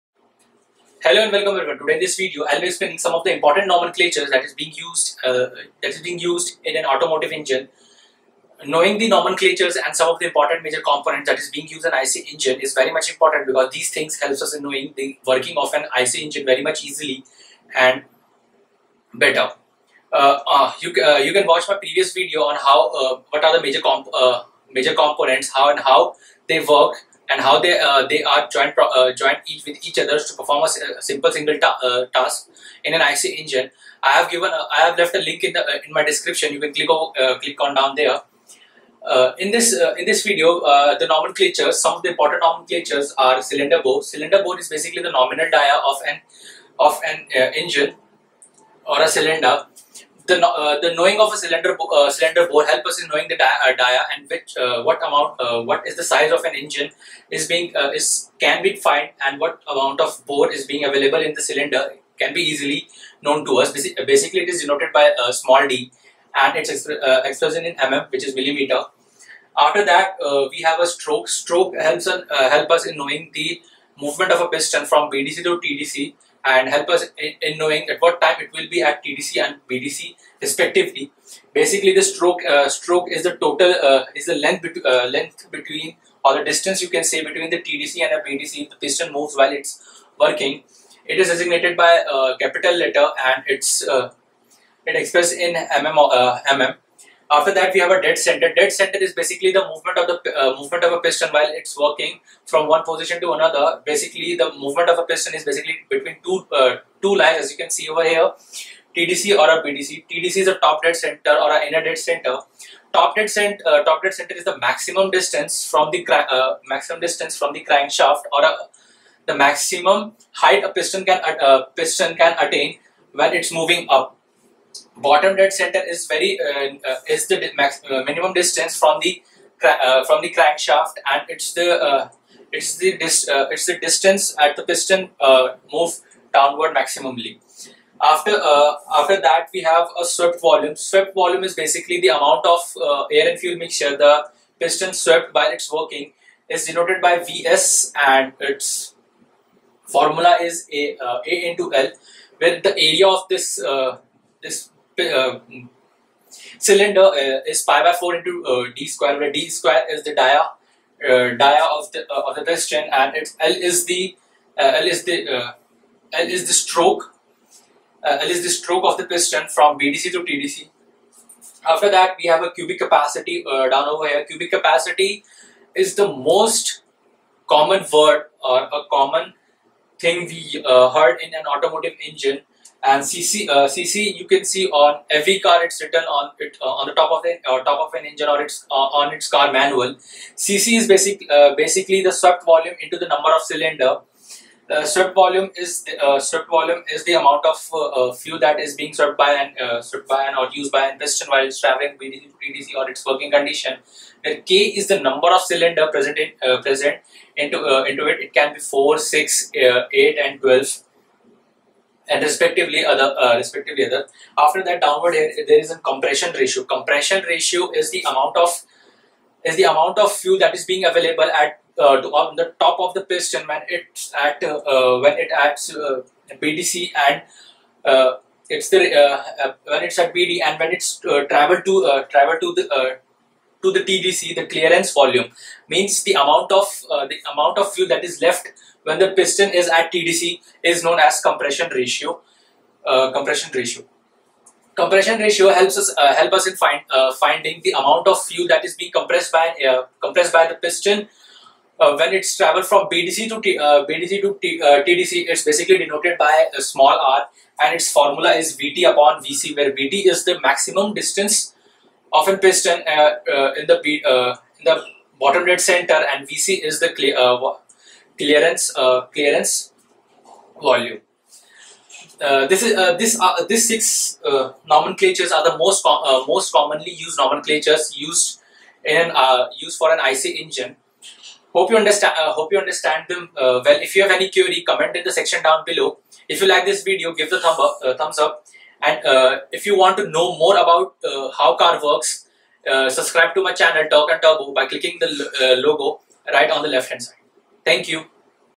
Nomenclature of IC engine || Explained !!
This video is about the nomenclatures of automobile that are basically use in while explaining the IC engine.. This video will help you getting through some of the most used nomenclature that are mostly use in Automobile industry..
Team TNT
Shubham ojha

This channel uploads videos in Hindi/English about automobile.